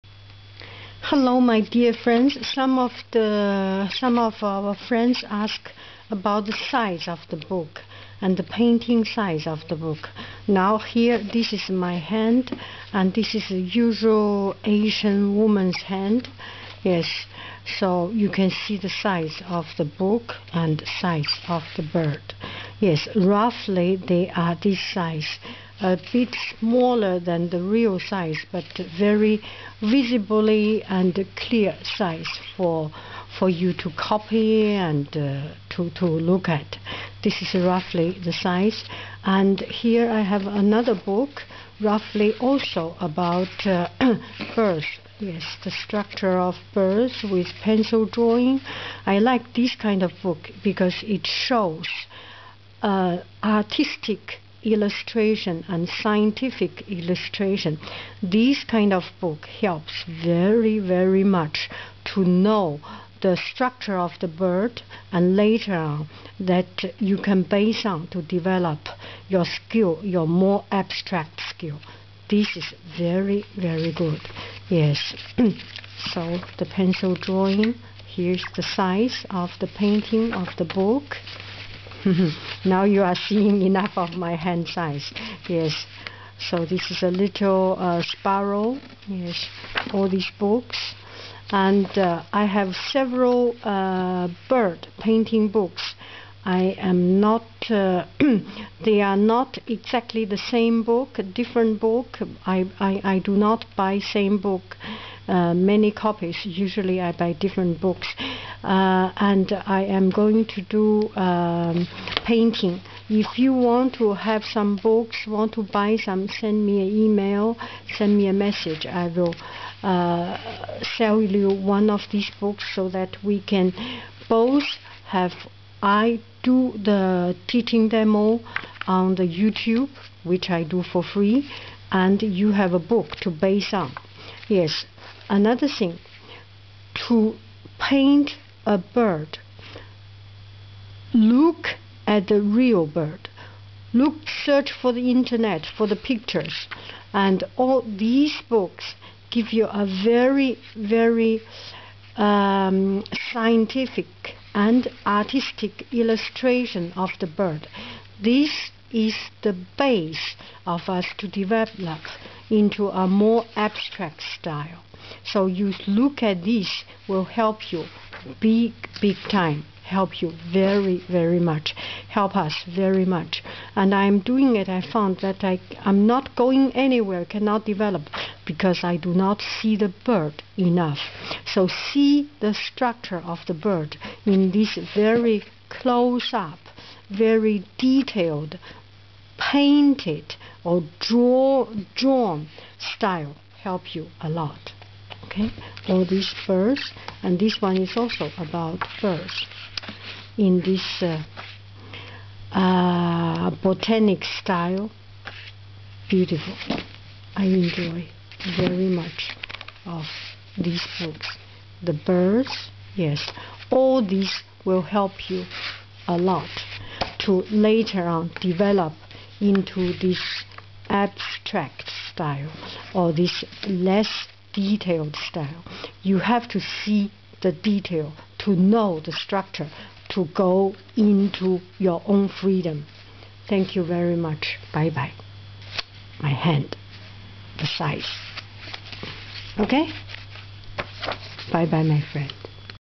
let's paint birds 1 - intro
I am planning to do bird painting demo, let me know your interest